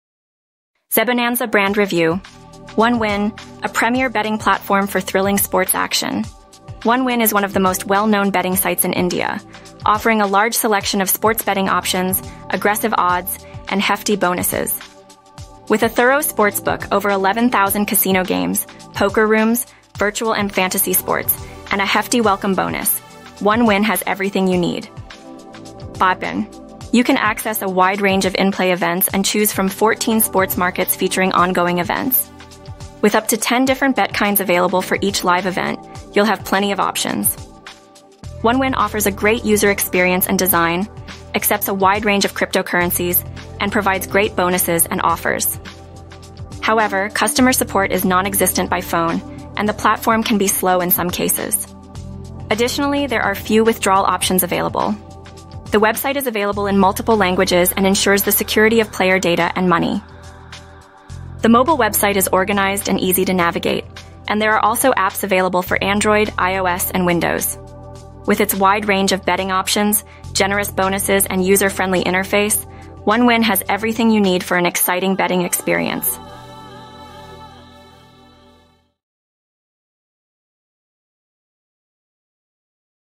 1Win (1जीत) India 2024 Introduction Video - India Bet Master
*IndiaBetMaster.com - 1Win India Brand Review*

An exclusive peek into 1Win India aimed specifically at the Indian user, and provides all the answers to the burning questions you may have before putting a bet on 1Win India.
What is 1Win? what is 1Win customer care number? do they have an app, and how to download and login to 1Win app? 1Win meaning, and much more. Please visit our brand review article using one of the links below (English or Hindi) for a comprehensive guide.